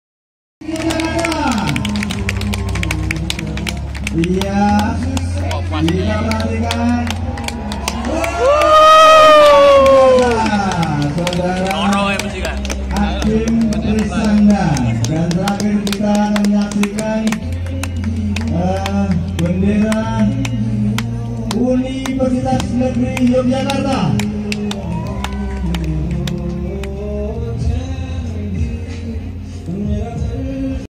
Terjun payung UNY 2017
Atraksi Terjun payung dalam rangka dies natalis UNY ke-53